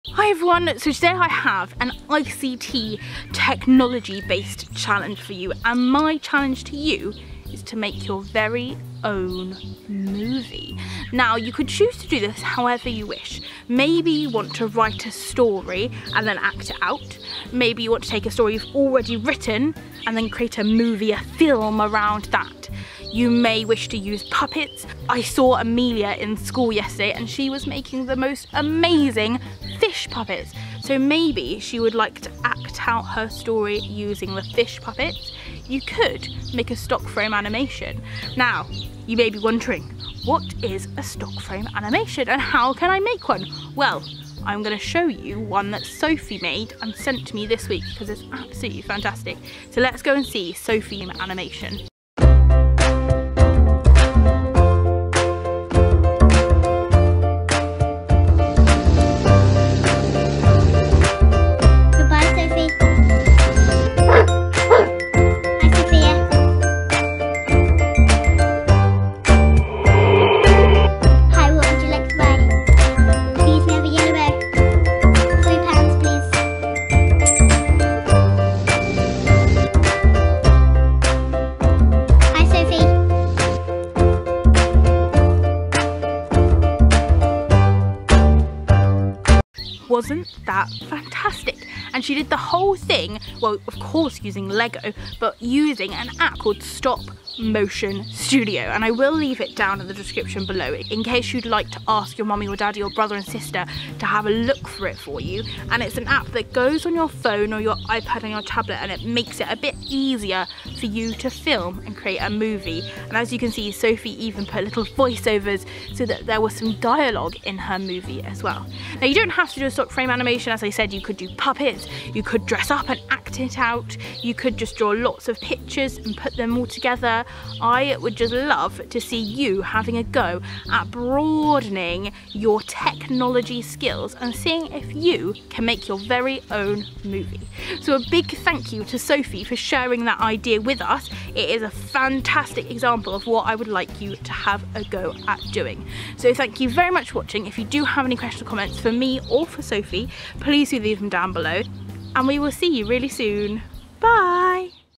Making a Movie - ft. Stop Frame Animation by Sophie
Sophie made this amazing film. Could you make a movie?
Best Wishes
Miss Soal